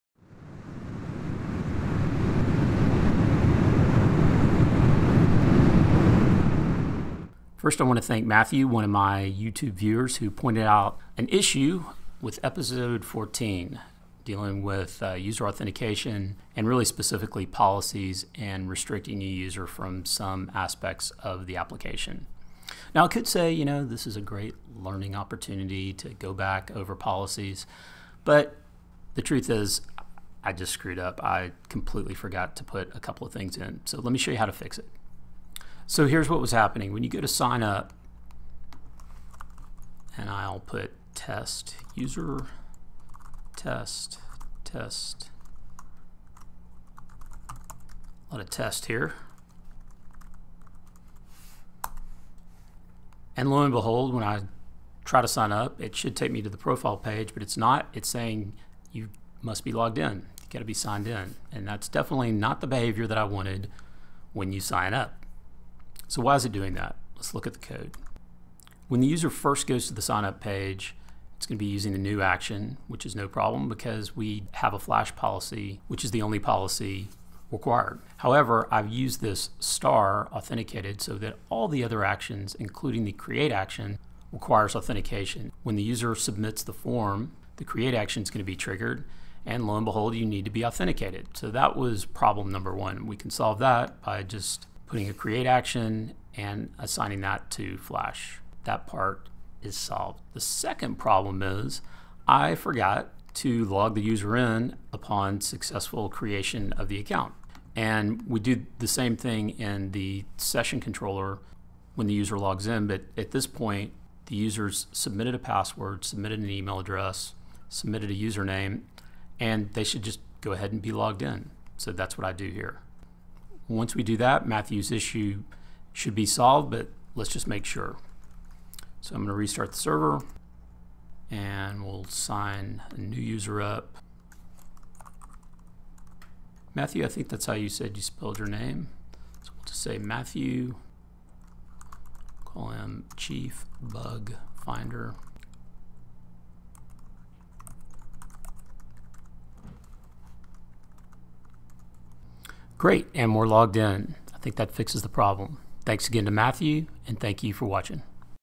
Building a Sails Application: Ep16 - Fixing an issue with policies in episode 14
This is a part of a series of screencasts which build a sails application from scratch.  The series will cover many important topics and features such as authentication, policies, real-time updates, databases, and much more.

To see all related videos to activityOverlord be sure and check out